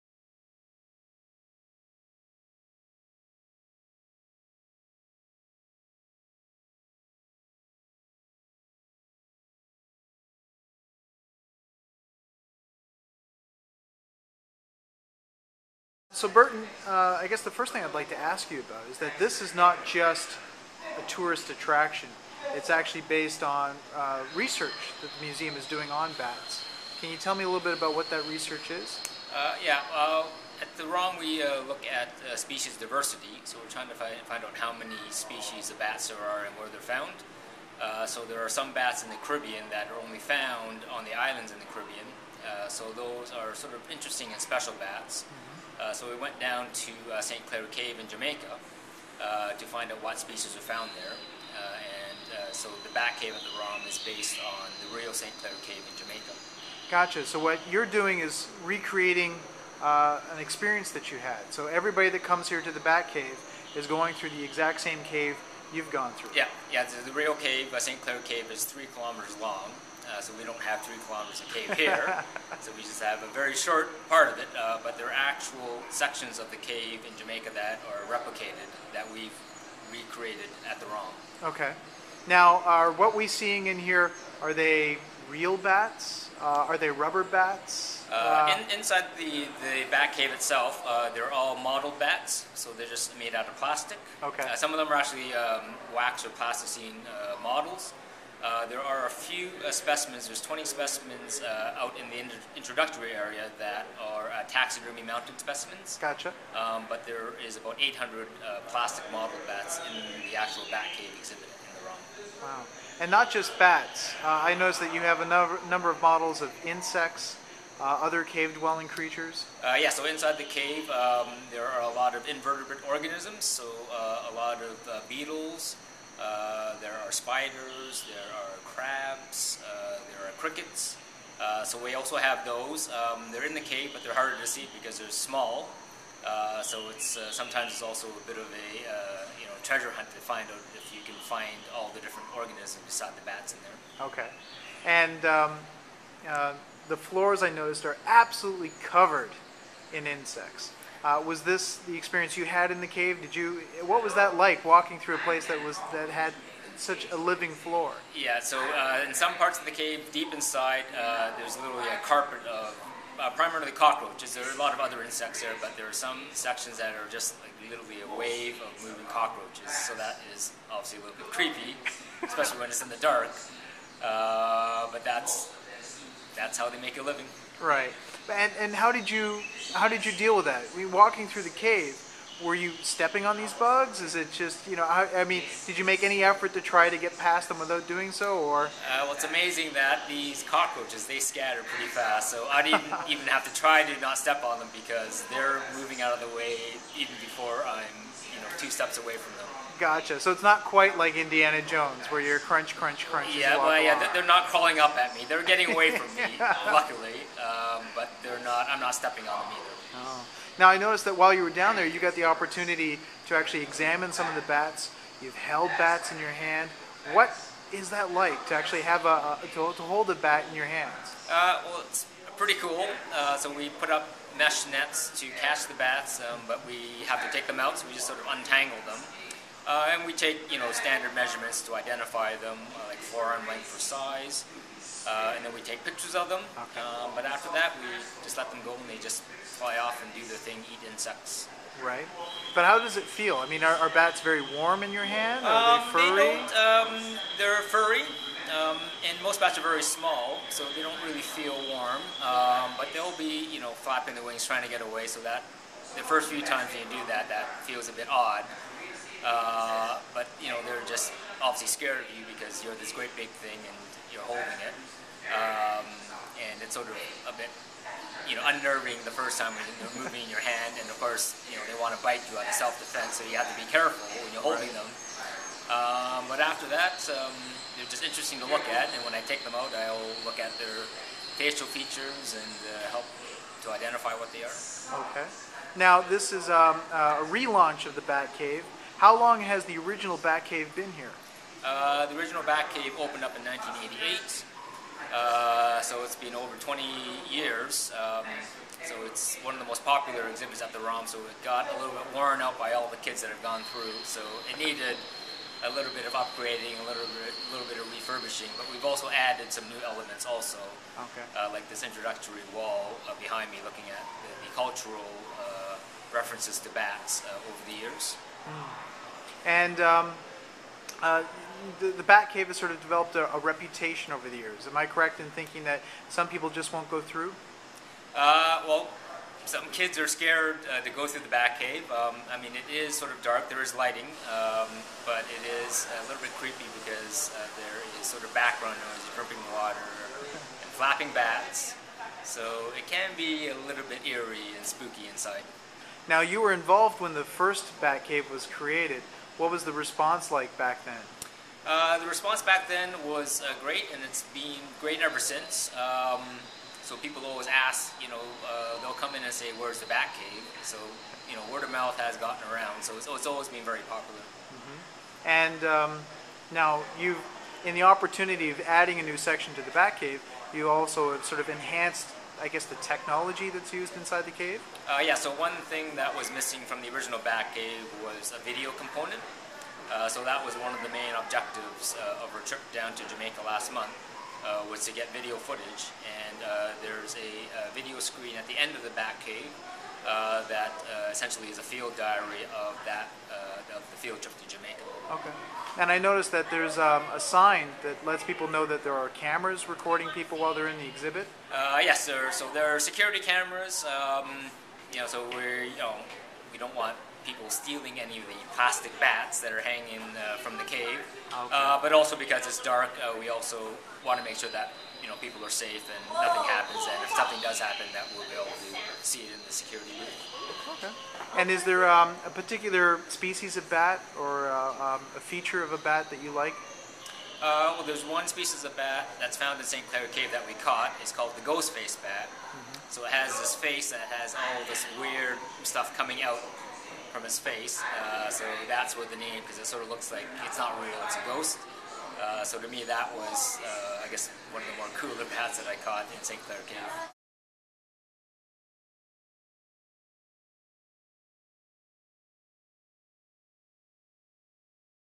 The Royal Ontario Museum's NEW Bat Cave
After more than two decades, the Royal Ontario Museum has completed a complete renovation of one of their most popular exhibits, the Bat Cave! On the eve of it's official re-opening CTV's Kris Abel chats with Assistant Curator of Mammalogy Burton Lim on the new changes and the field research behind the new renovation.